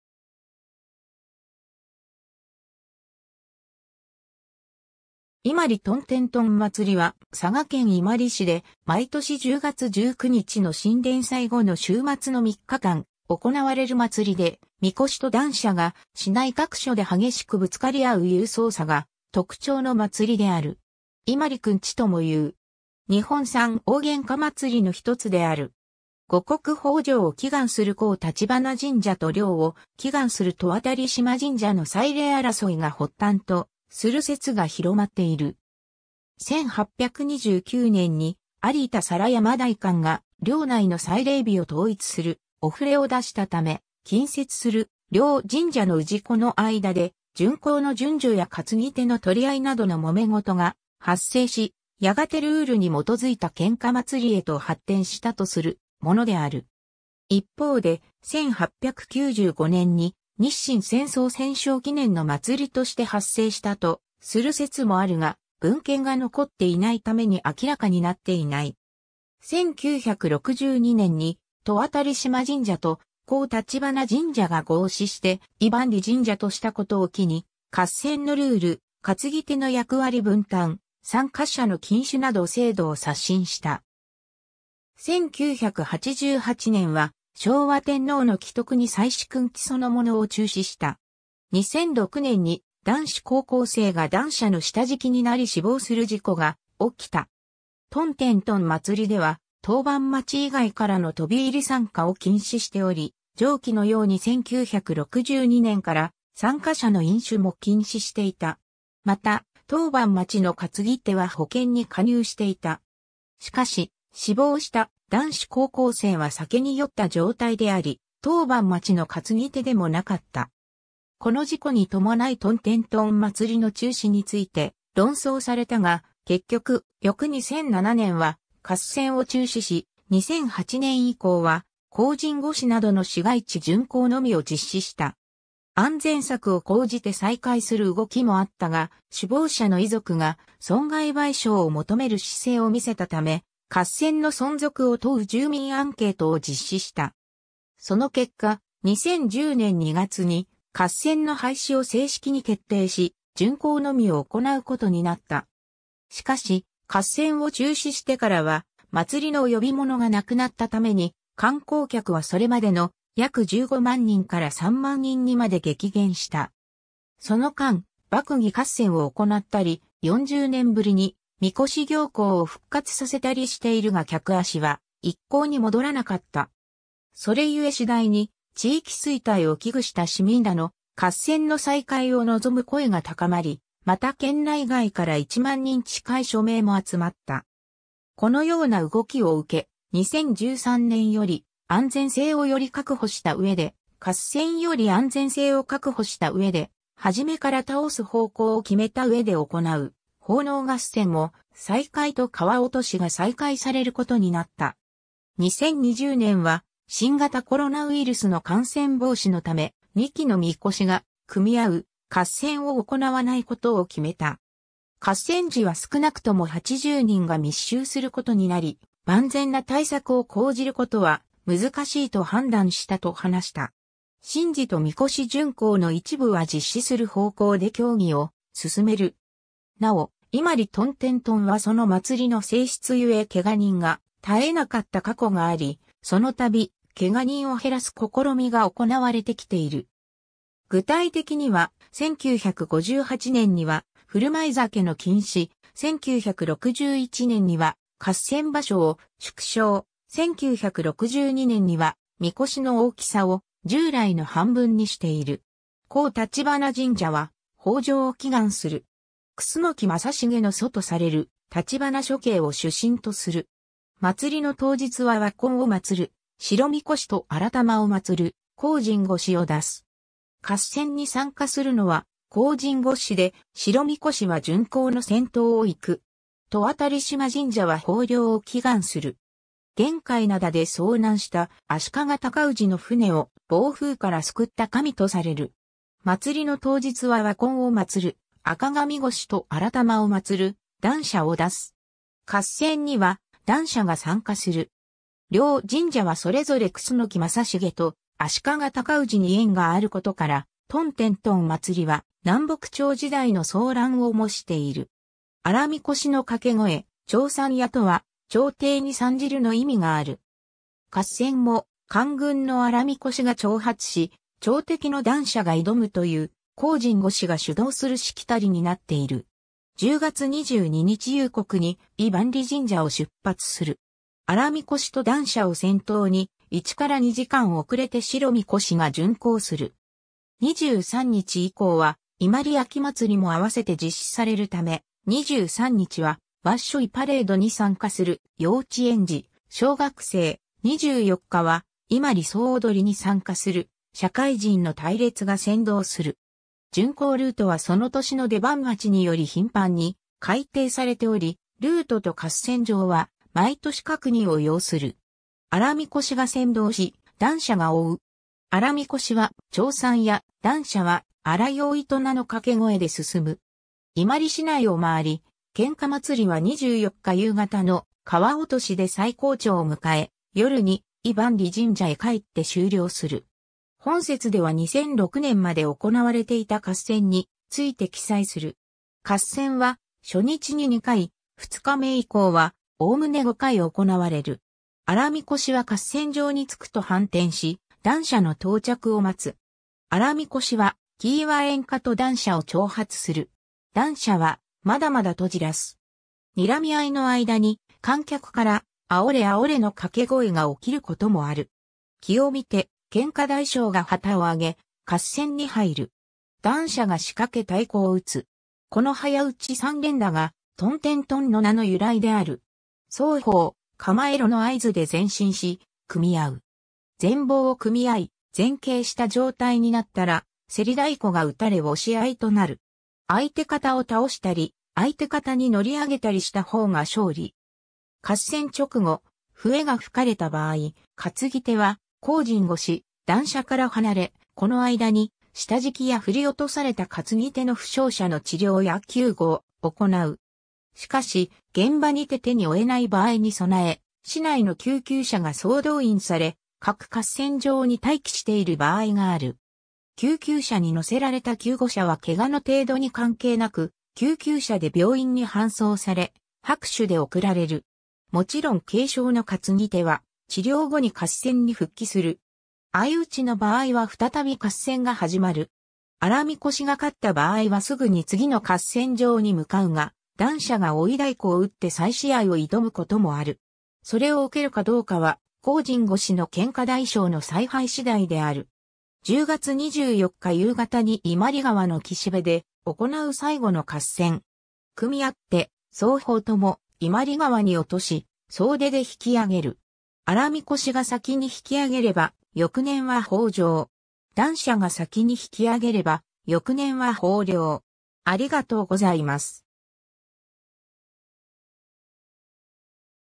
伊万里トンテントン祭り
伊万里トンテントン祭り（いまりトンテントンまつり）は、佐賀県伊万里市で毎年10月19日の神殿祭後の週末の3日間行われる祭で、神輿（みこし）と団車（だんじり）が市内各所で激しくぶつかり合う勇壮さが特徴の祭である。
伊万里くんちとも言う。
日本三大喧嘩祭りの一つである。
 五穀豊穣を祈願する香橘神社（こうきつじんじゃ）と漁を祈願する戸渡嶋神社（ととしまじんじゃ）の祭礼争いが発端とする説が広まっている。
1829年（文政12年）に有田皿山代官が領内の祭礼日を統一する御触を出したため、近接する両神社の氏子の間で巡行の順序や担ぎ手の取り合いなどの揉め事が発生し、やがてルールに基づいた喧嘩祭りへと発展したとするものである。
一方で、1895年（明治28年）に日清戦争戦勝記念の祭りとして発生したとする説もあるが、文献が残っていないために明らかになっていない。
 1962年（昭和37年）に戸渡嶋神社と香橘神社が合祀して伊萬里神社（いまりじんじゃ）としたことを機に、合戦のルール、担ぎ手の役割分担、参加者の禁酒など制度を刷新した。
 1988年（昭和63年）は昭和天皇の危篤に際しくんちそのものを中止した。
 2006年（平成18年）に、男子高校生（当時17歳）が団車の下敷きになり死亡する事故が起きた。
トンテントン祭りでは「当番町以外からの飛び入り参加」を禁止しており、上記のように1962年（昭和37年）から「参加者の飲酒」も禁止していた。
また、当番町の担ぎ手は保険に加入していた。
しかし、死亡した男子高校生は酒に酔った状態であり、当番町の担ぎ手でもなかった。
 この事故に伴いトンテントン祭りの中止について論争されたが、結局翌2007年（平成19年）は合戦を中止し、2008年（平成20年）以降は荒神輿などの市街地巡行のみを実施した。
安全策を講じて再開する動きもあったが、死亡者の遺族が損害賠償を求める姿勢を見せたため、合戦の存続を問う住民アンケートを実施した。
その結果、2010年（平成22年）2月に合戦の廃止を正式に決定し巡行のみを行うことになった。
しかし、合戦を中止してからは、祭りの呼び物がなくなったために、観光客はそれまでの約15万人から3万人にまで激減した。
その間、摸擬合戦を行ったり、40年ぶりに神輿行幸を復活させたりしているが客足は一向に戻らなかった。
それゆえ次第に、地域衰退を危惧した市民らの、合戦の再開を望む声が高まり、また県内外から10000人近い署名も集まった。
このような動きを受け、2013年（平成25年）より安全性をより確保した上で、合戦（2013年は倒さずに引き太鼓で納める摸擬合戦のみ、2014年（平成26年）より安全性を確保した上で、初めから倒す方向を決めた上で行う奉納合戦も再開）と川落とし（夜から夕方に変更）が再開されることになった。
 2020年（令和2年）は新型コロナウイルスの感染防止のため、2基の神輿が組み合う「合戦」を行わないことを決めた。
「合戦時は少なくとも80人が密集することになり、万全な対策を講じることは難しいと判断した」と話した。
神事と神輿巡行の一部は実施する方向で協議を進める。
 なお、伊万里トンテントンはその祭りの性質ゆえけが人が絶えなかった過去があり、そのたび、けが人を減らす試みが行われてきている。
具体的には、1958年には振る舞い酒の禁止、1961年には合戦場所を縮小、1962年には神輿の大きさを従来の半分にしている。
 香橘神社は豊穣を祈願する。
楠木正成の祖とされる橘諸兄を主神とする。
祭りの当日は和魂を祀る「白神輿」と荒魂を祭る「荒神輿」を出す。
合戦に参加するのは荒神輿で、白神輿は巡行の先頭を行く（白神輿、赤神輿、荒神輿、団車の順で巡行を行う）。
 戸渡嶋神社は豊漁を祈願する。
玄界灘で遭難した足利尊氏の船を暴風から救った神とされる。
祭りの当日は和魂を祀る「赤神輿」と荒魂を祭る「団車」を出す。
合戦には団車が参加する。
 両神社はそれぞれ楠木正成と足利尊氏に縁があることから、トンテントン祭りは南北朝時代の争乱を模している。
荒神輿の掛け声「チョーサンヤ」とは「朝廷に参じる」の意味がある。
合戦も官軍の荒神輿が挑発し、朝敵の団車が挑むという、荒神輿が主導するしきたりになっている。
 10月22日夕刻に伊萬里神社を出発する。
荒神輿と団車を先頭に、1 - 2時間遅れて白神輿が巡行する。
23日以降は「伊万里秋祭り」も併せて実施されるため、23日は「わっしょいパレード」に参加...